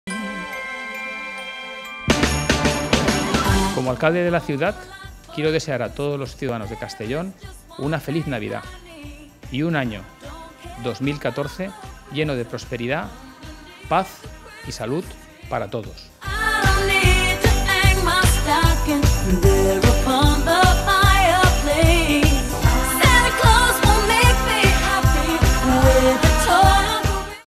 Felicitación del Alcalde de Castellón Alfonso Bataller
Felicitación de Navidad 2013 del Alcalde de Castellón Alfonso Bataller